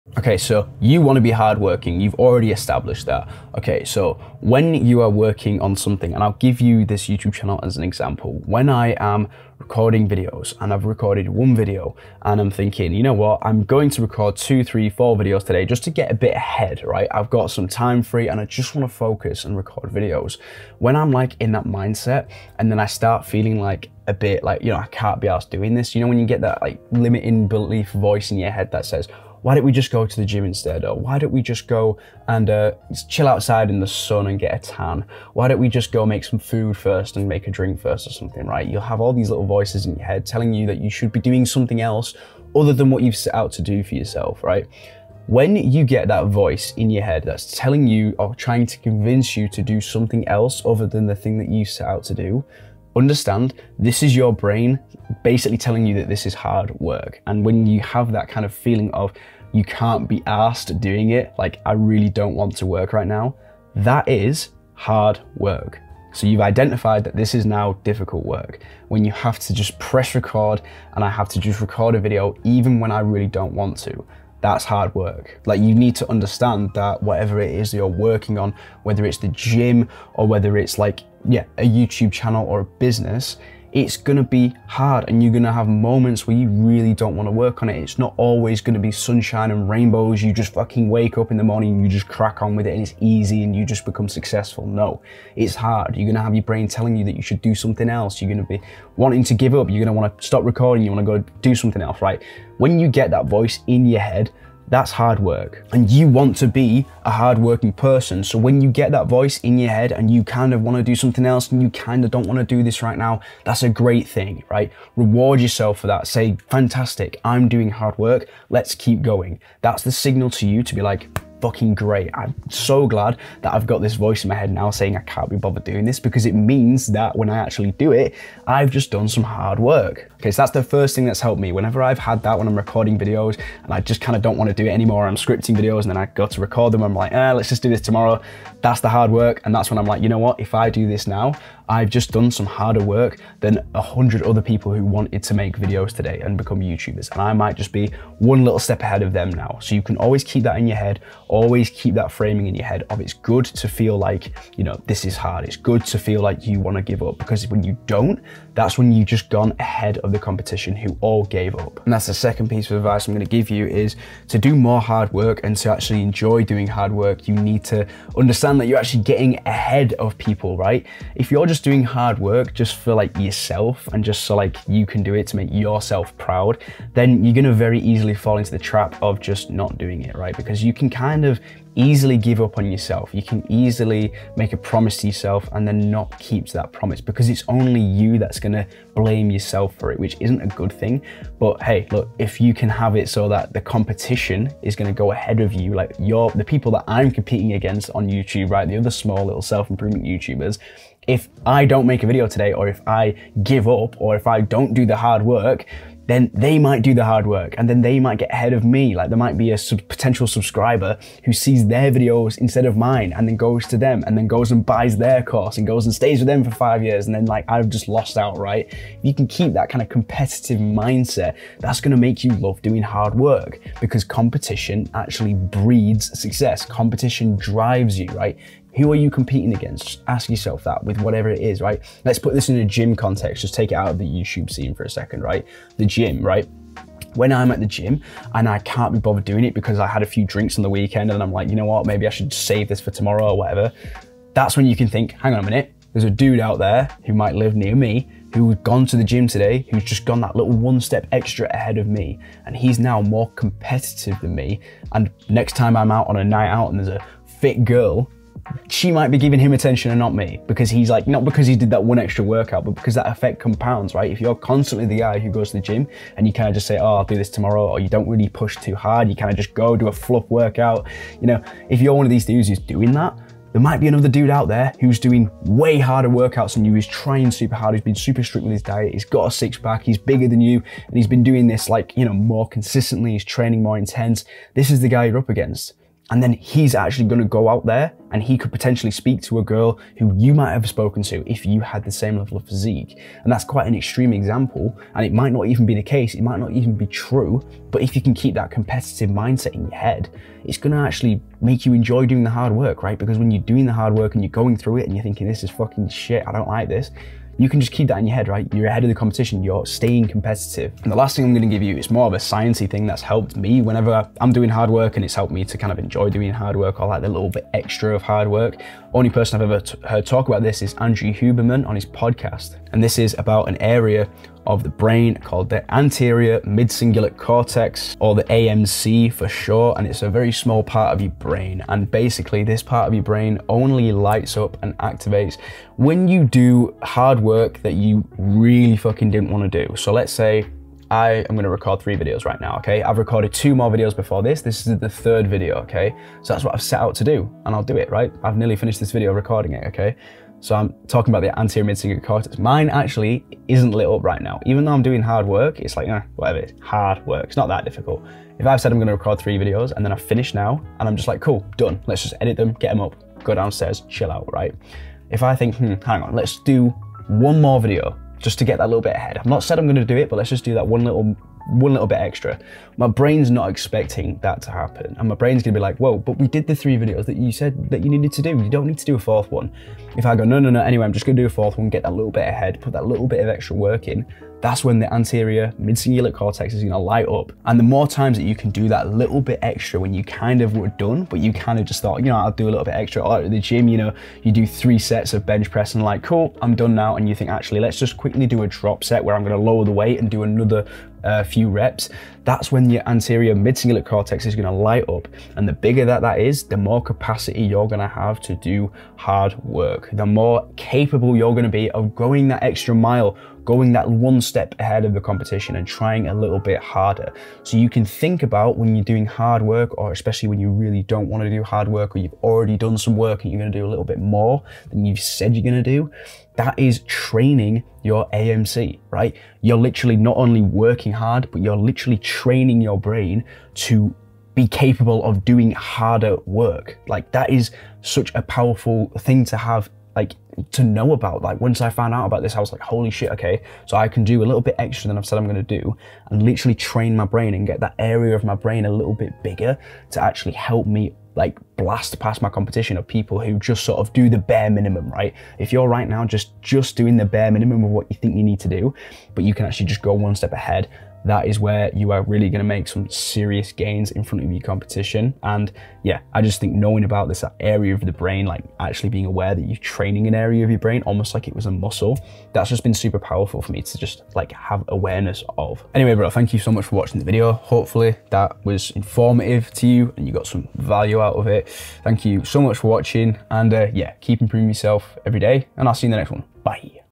How To Trick Your Brain Into LOVING Hard Work
In this video I'll give you 3 tips on how you can trick your brain in to loving doing hard work rather than seeing it as a chore.